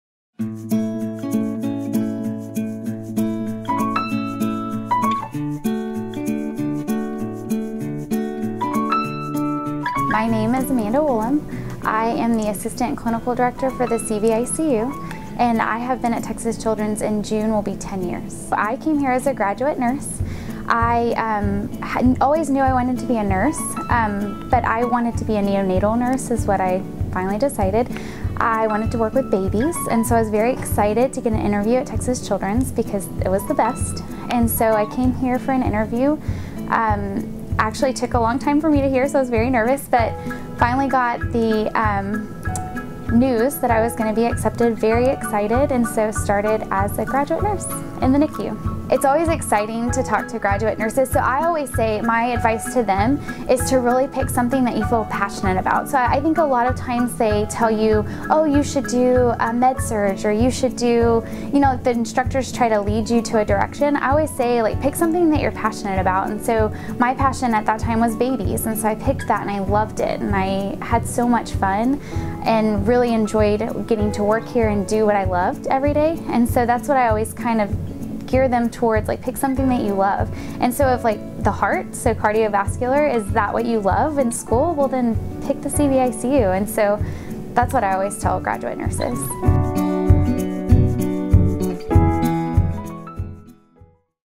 Finding Your Passion - Amanda Wollam
Amanda Wollam began at Texas Children's as a graduate nurse in the NICU. Now, as an Assistant Clinical Director in the CVICU, she enjoys driving new graduate nurses towards finding their passions.